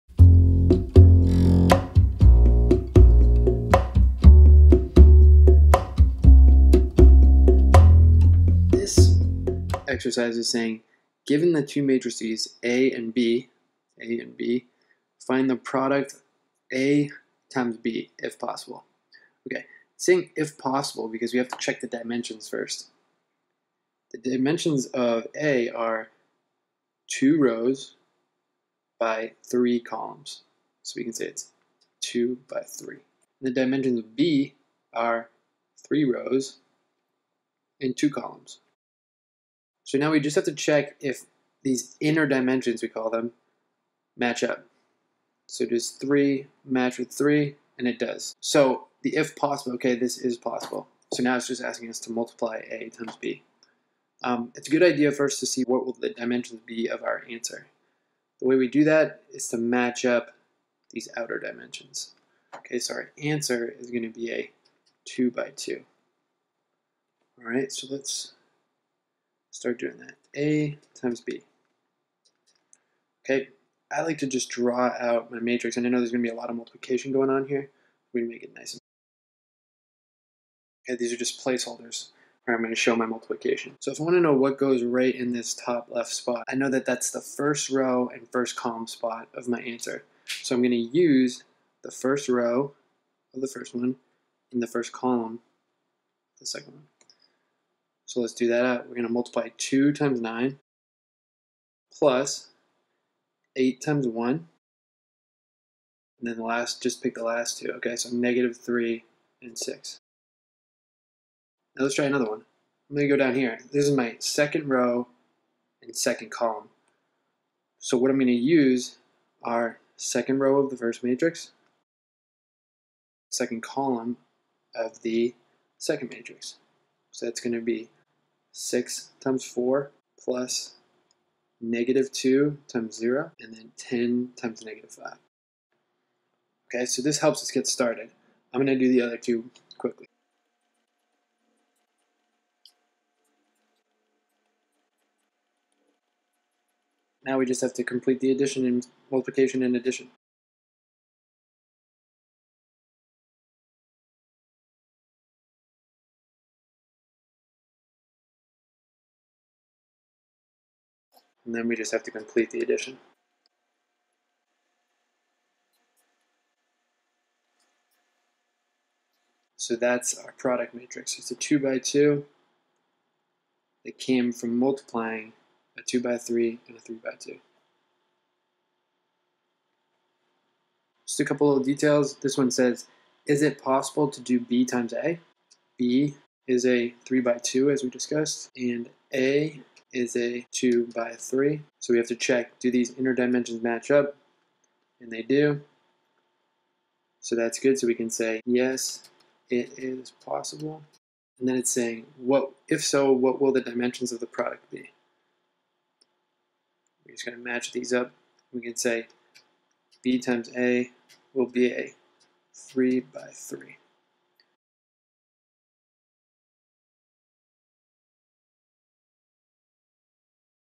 Multiplying matrices by hand
This video will help you understand how to multiply two matrices together without using your calculator. The video also helps you understand the dimensions of matrices and when you can multiply two matrices.